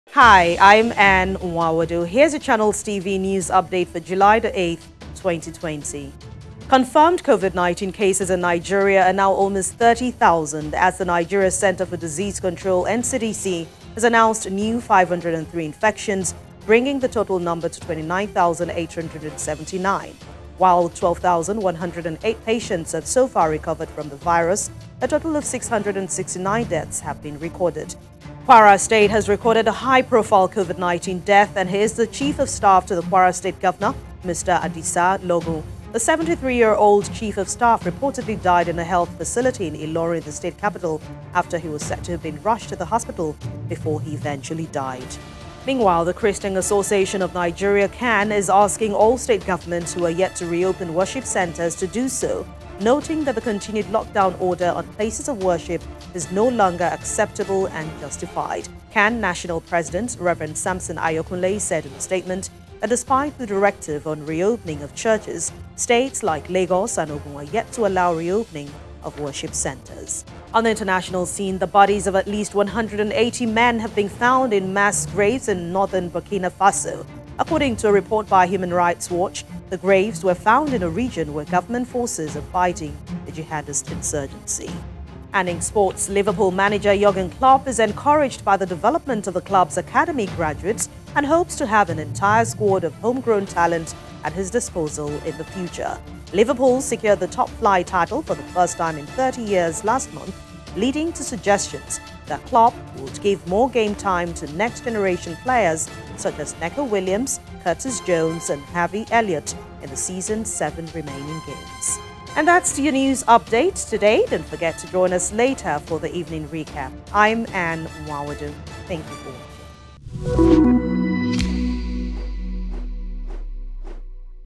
Update: CAN Seeks Reopening Of Churches
The Christian Association of Nigeria (CAN), is asking all state governments who are yet to reopen worship centres to do so, noting that the continued lockdown order on places of worship is no longer acceptable and justified.

CAN National President, Rev. Samson Ayokunle said in a statement that despite the directive on reopening of churches, states like Lagos and Ogun are yet to allow reopening of worship centres.

Start your morning updated and end the evenings with a recap of the day's headlines.